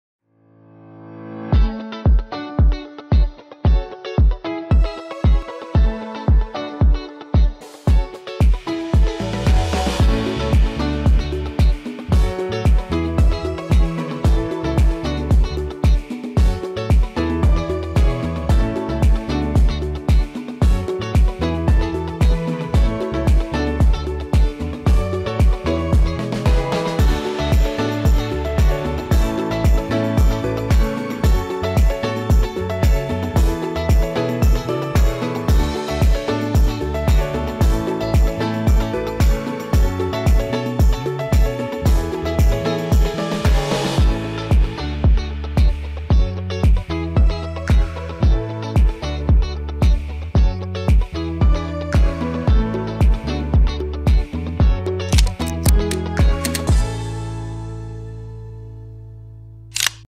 7. Triangle | How to Make a Model Triangle with Click Builders Building Toy by SUPA STEM
Find out how to make a triangle with Click Builders construction toy!